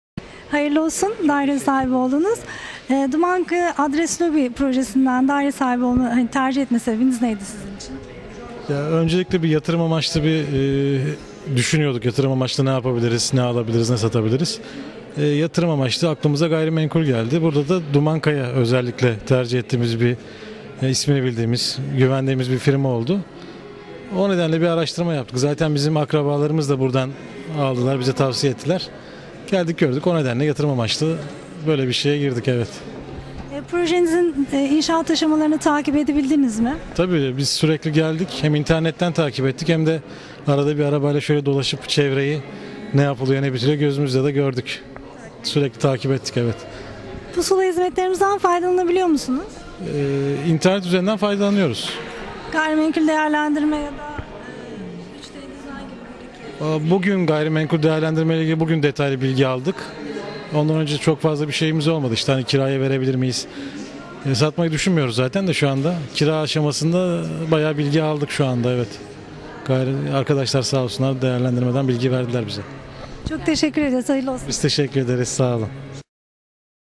Dumankaya Adres Lobi Projemizin Daire Teslim Röportajları - 9
Tuzla'da gerçekleştirdiğimiz Dumankaya Adres Lobi projemizde daire teslimleri tamamlandı. Daire sahiplerimizin görüşlerini sizlerle paylaşmaktan keyif duyuyoruz.